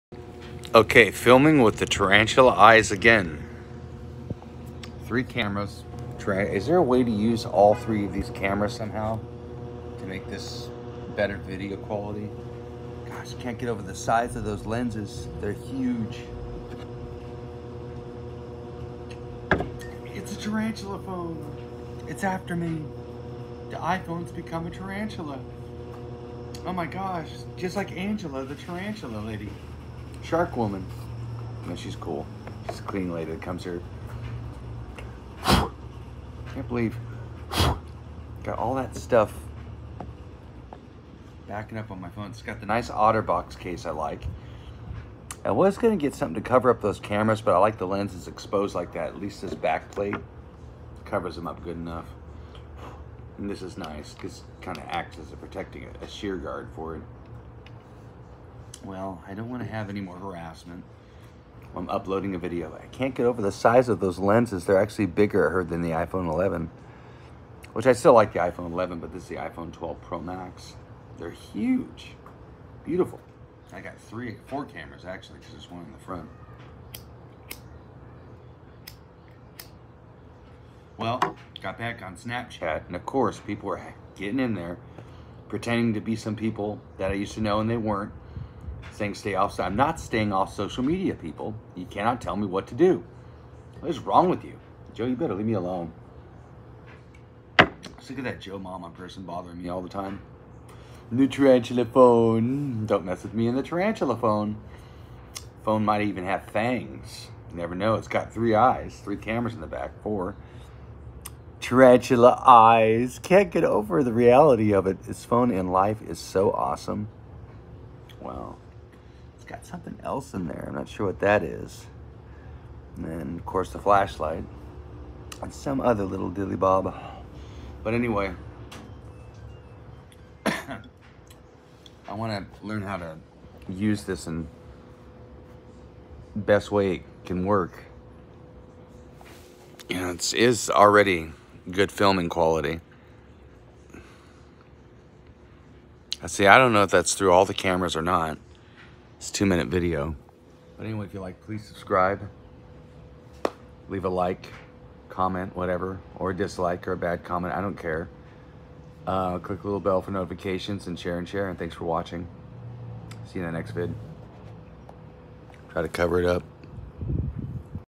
Tarantula phone
In the video I was talking about the cameras on my iPhone 12 Pro Max how they look like tarantulas I decided his name at the tarantula phone